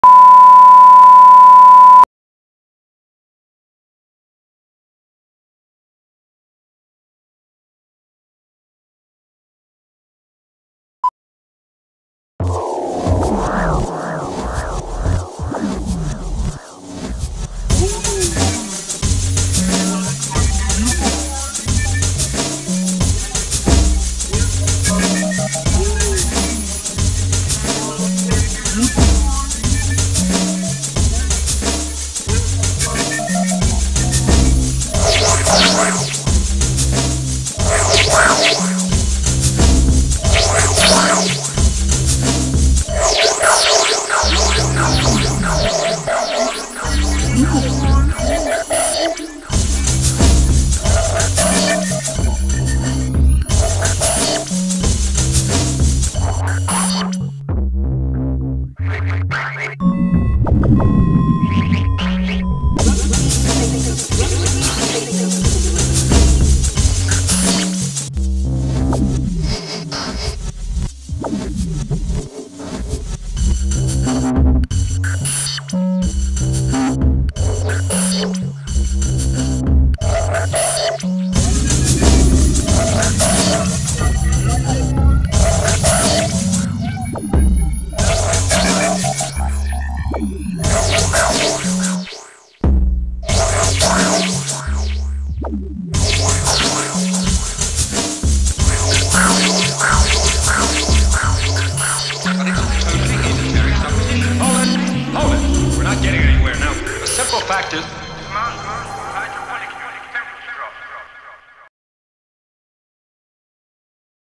FinalEditNewSound
The C.O.R.E. Vision showreel from 1998. I can't remember the venue at which most of the footage was filmed, perhaps St Matthew's Church in Brixton, and I have no idea who the DJ is that features part way through. Still it is a reasonable record of the spaces and environments C.O.R.E. were able to create with slide, 8mm, 16mm, video and UV backdrops. The homemade soundtrack is pretty cool too...